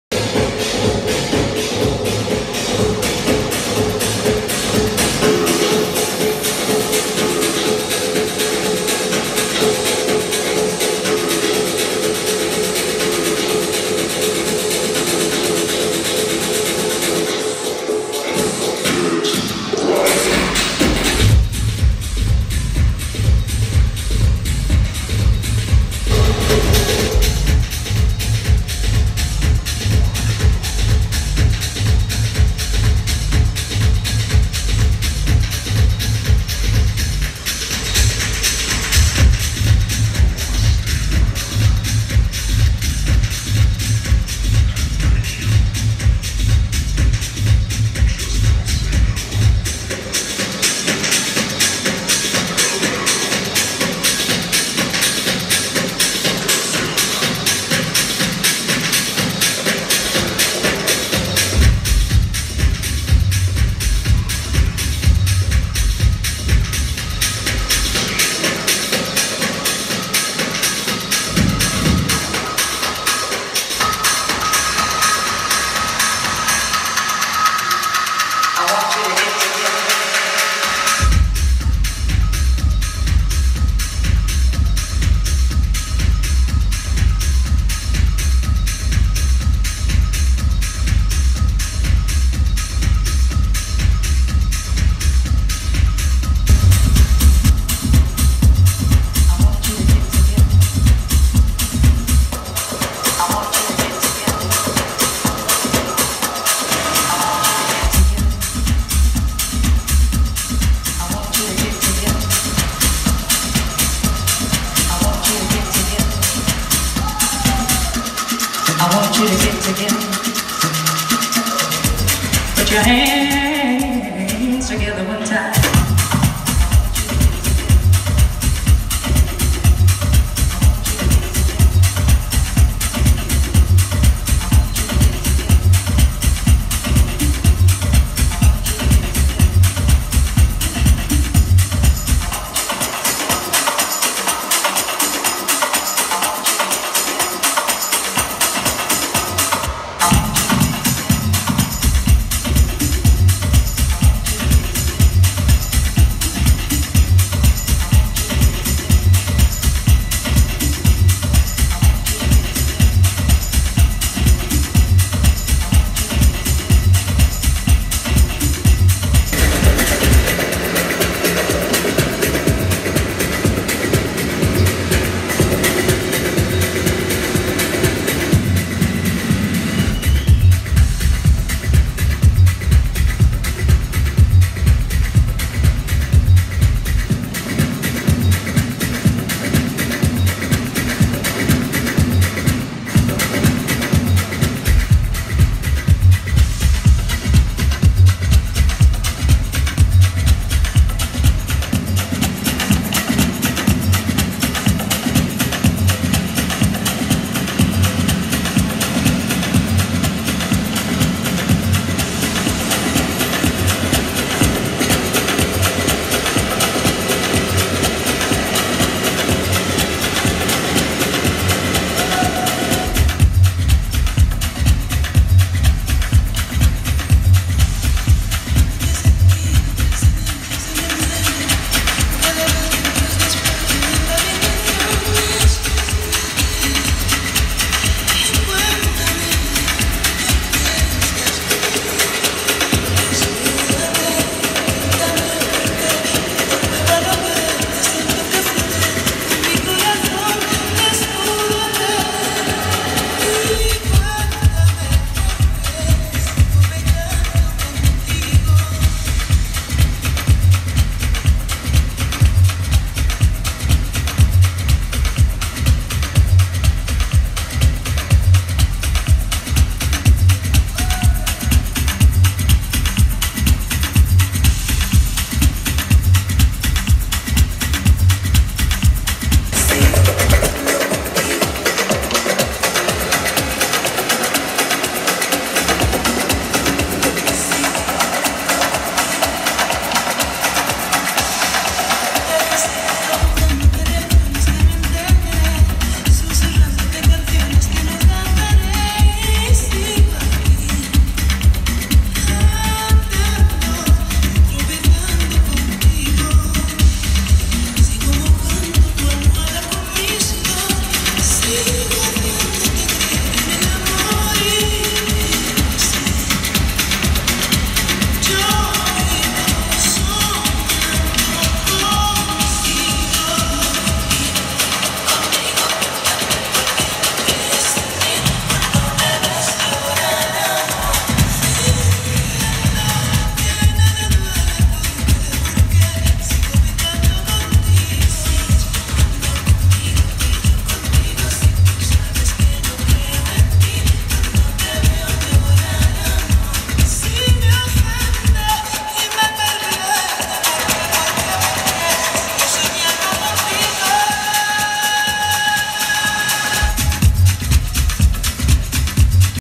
Marcos Silvestre@Stark (La Trilogía 2108) "Sala Lab Madrid"
Marcos Silvestre en la fiesta de la trilogía de StarkSensation en la conocida sala "Lab" (Estación de Chamartin), compartiendo cartel con artistas como Oscar de Rivera o Ismael Rivas.